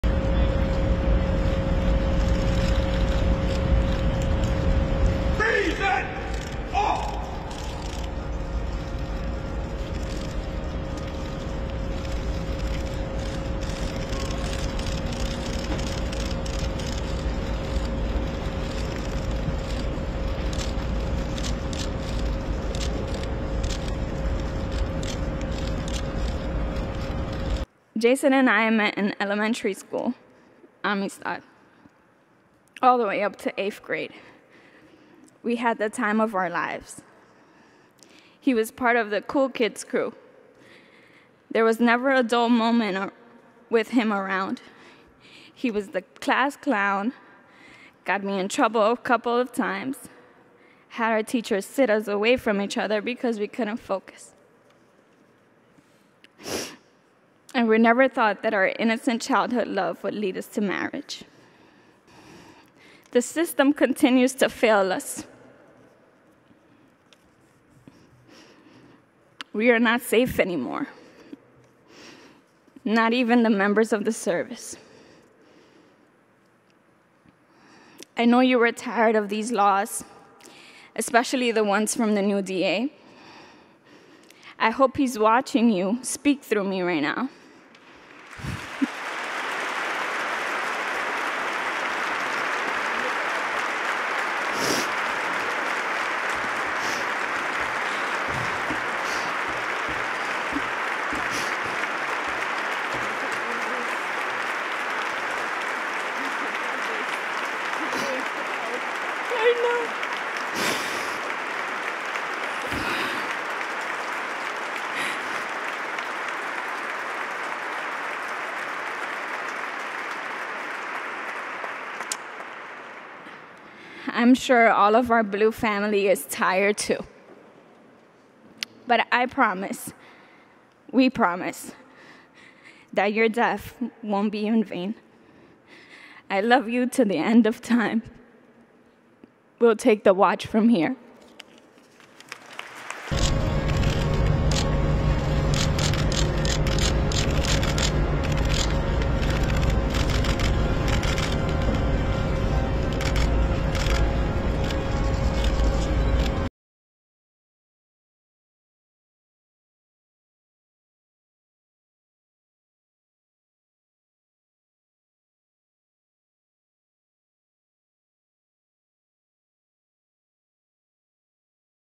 Killed officer's widow lashes out at Manhattan DA
Dominique Luzuriaga, the wife of slain NYPD police officer Jason Rivera, lashed out at the new Manhattan District Attorney at her husband's funeral on Friday. (Jan. 28)